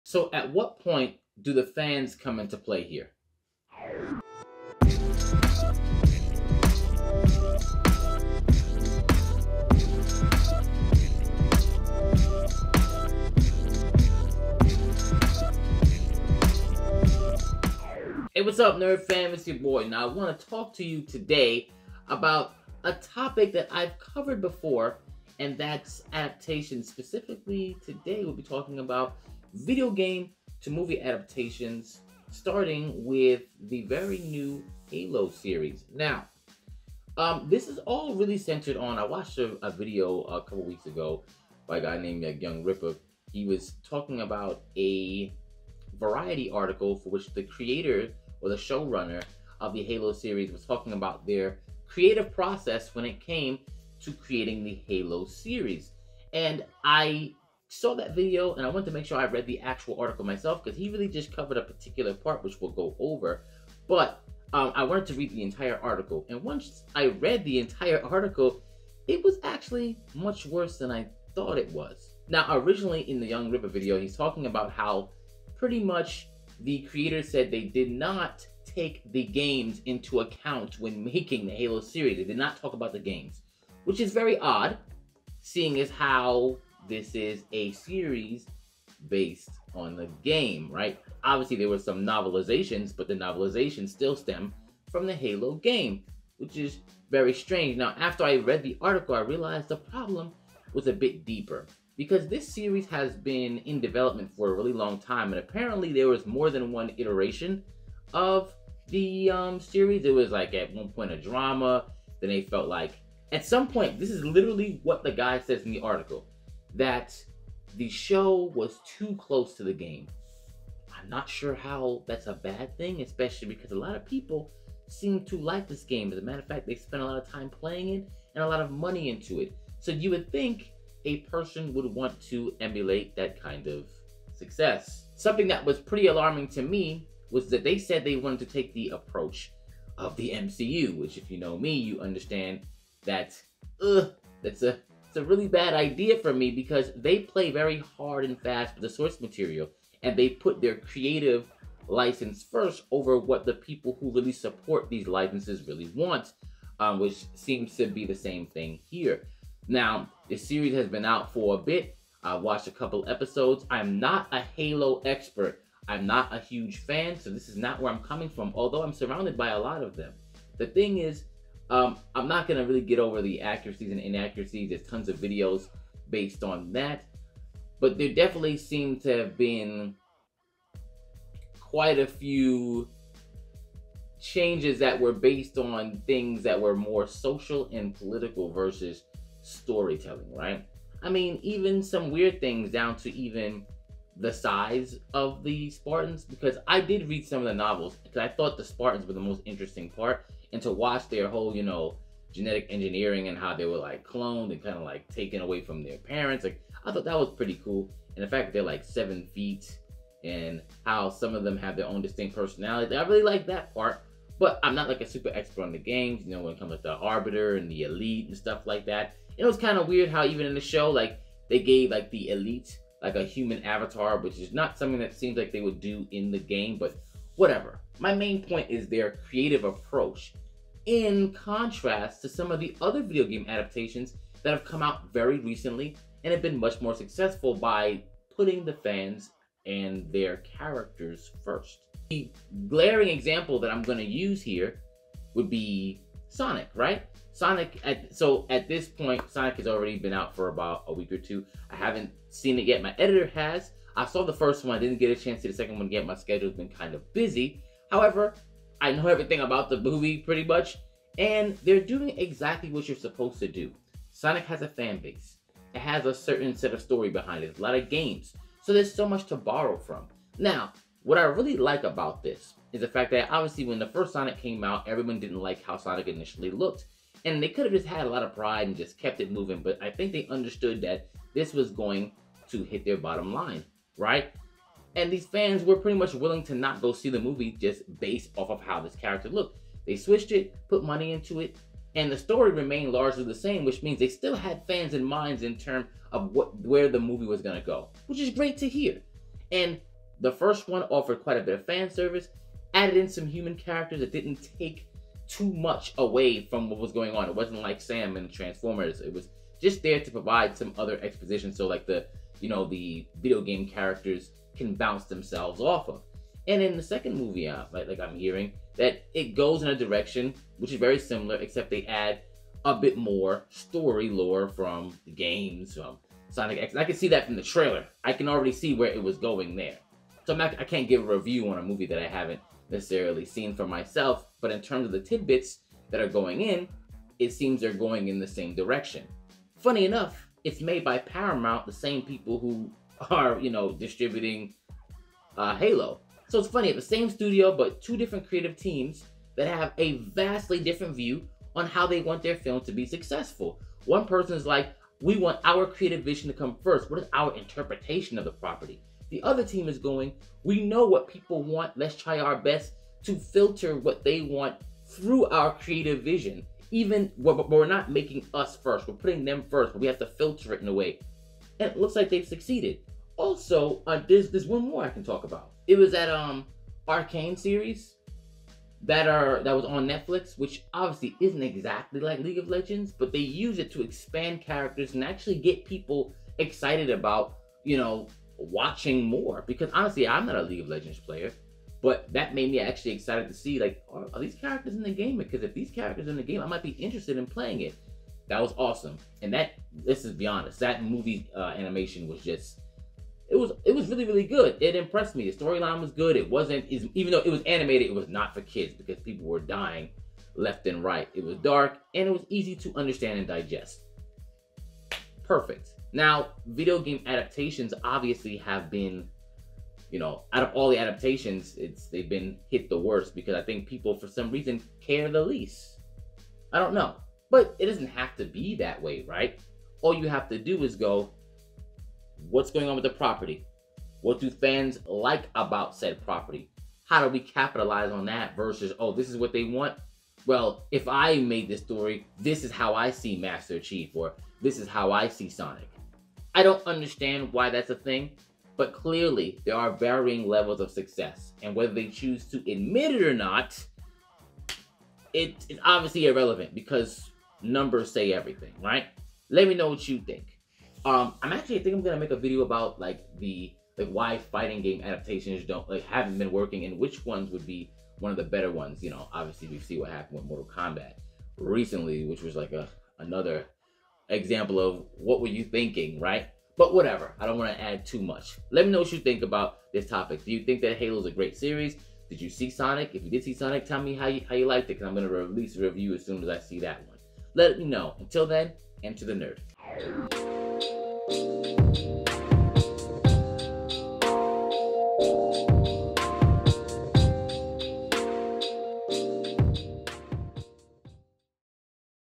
Video Game Adaptations Are Better When You Put The Fans First.
As we know the Halo T.V. series is out. The creative process behind it was one devoid of catering to the fans. This is very evident if you have seen the show. However Sonic 2 which is another Video Game adaptation,  albeit in film form. Does a much better Job at catering to what the core fans of the franchise would appreciate. Let's talk about it!